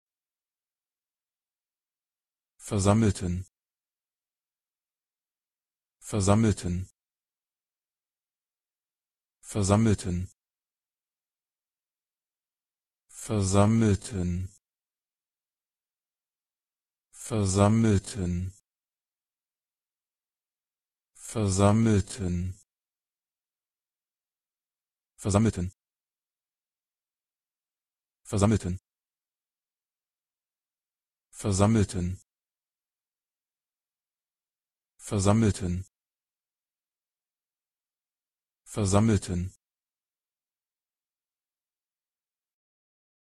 How to pronounce versammelten in German - Perfectly
This video shows you how to pronounce versammelten in German , pronounce and pronunciation how-to guide.
Watch and learn  how to say this word rightly with my channel, how to pronounce versammelten  free pronunciation videos.

What does this versammelten mean ? Words’ meaning, dictionary description, information, explanation.
gathered, assembled, collected, congregated, rallied, came together, would gather, congregation, were gathering, met, meeting, crowd, convened

1 .EN They gathered(versammelten) the whole collection, and family stories of these dolls.
1 .DE Sie versammelten(gathered) die ganze Sammlung und Familie Geschichten dieser Puppen.

2 .EN By noon, dozens of neighbours had gathered(versammelten).
2 .DE Mittags versammelten(gathered) sich Dutzende von Anwohnern.

3 .EN Large numbers of people assembled(versammelten) all along the route.
3 .DE Entlang des Weges versammelten(assembled) sich viele Menschen.

00:00 - Normal versammelten  Pronounce
00:10 - How to pronounce versammelten ?